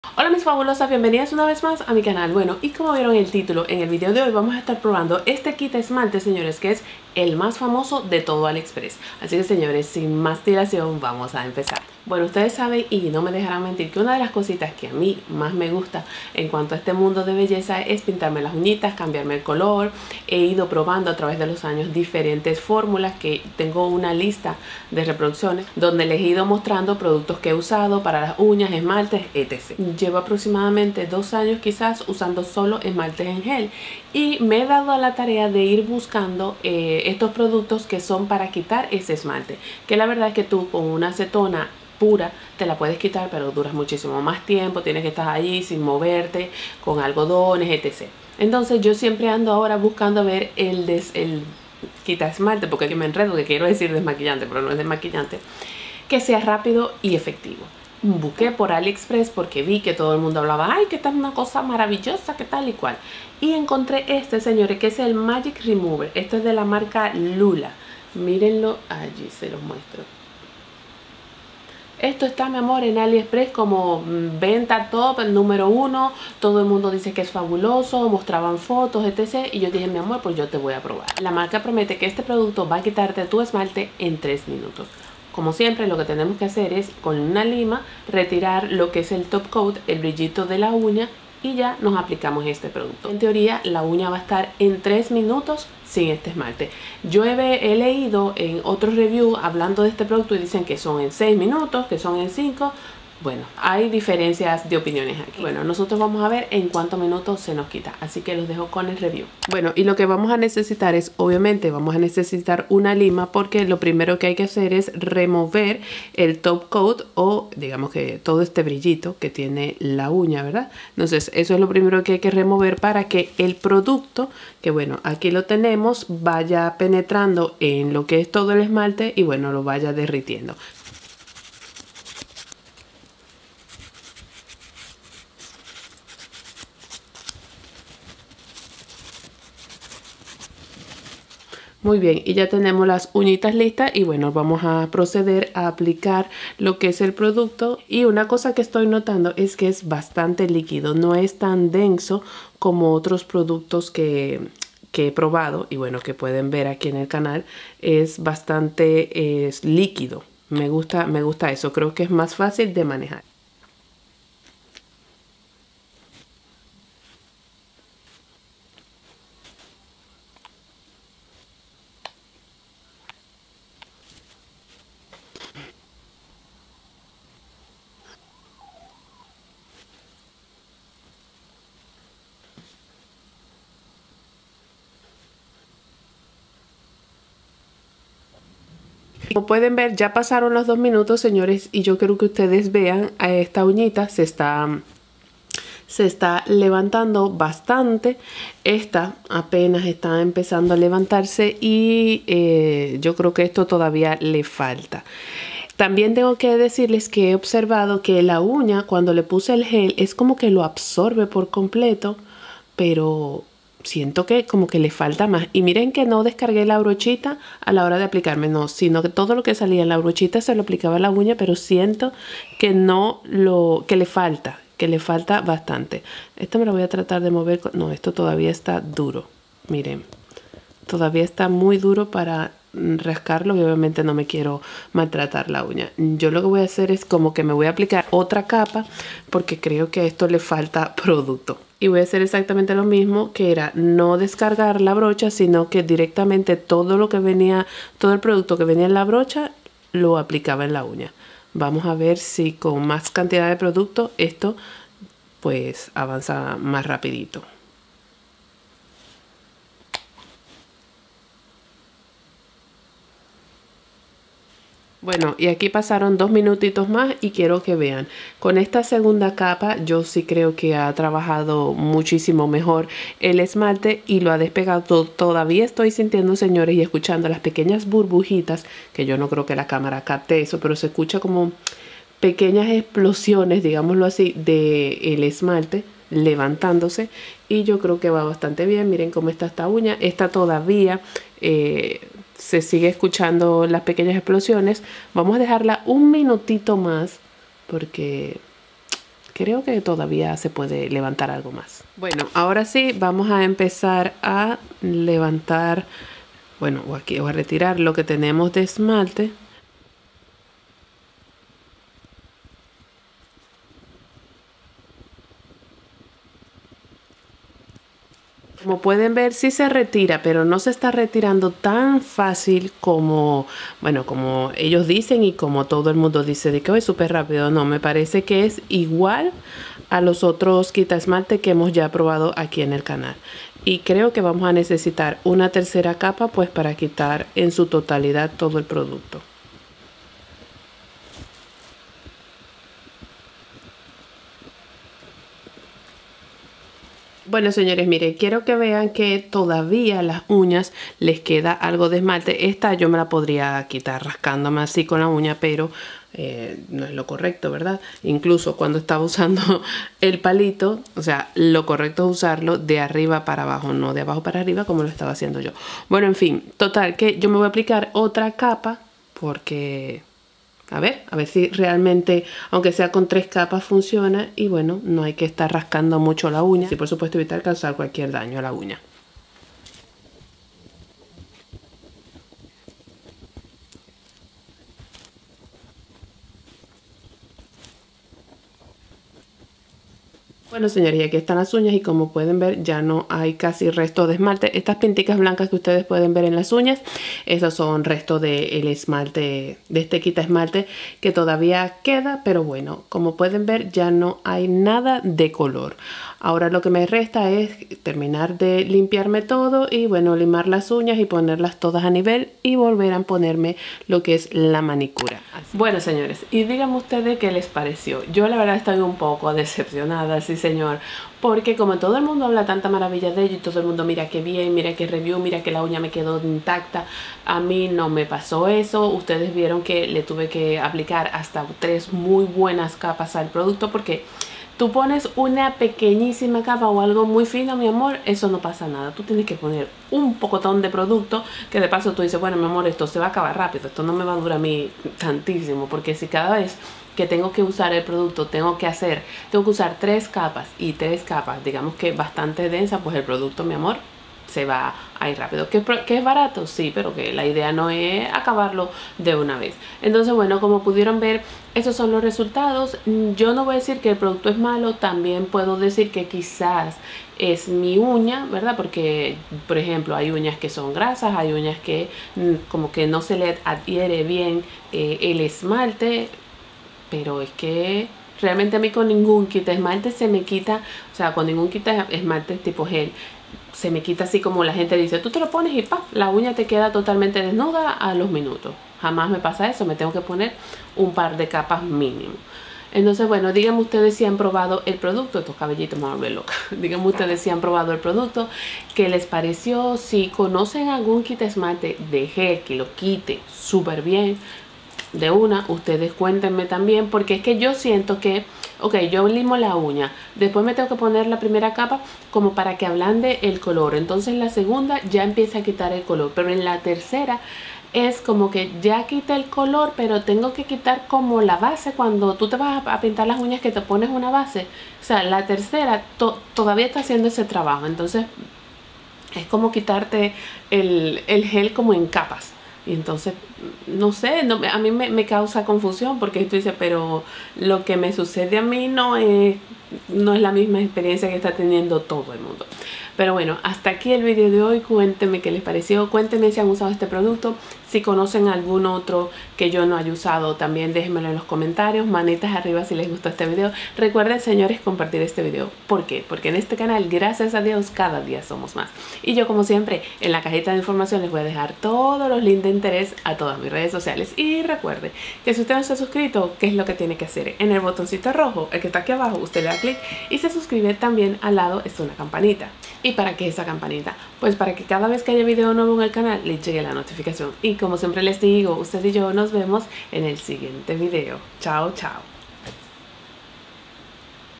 MAGIC REMOVER GEL | EL MÁS FAMOSO EN TODO ALIEXPRESS | NINA
Mis Fabs hoy les muestro este removedor de esmalte permanente de @AliExpressGlobal  y lo ponemos a prueba.

ADDRESS
Ninoska
620 Centerpoint Blvd
United States

VIDEOS RECOMENDADOS.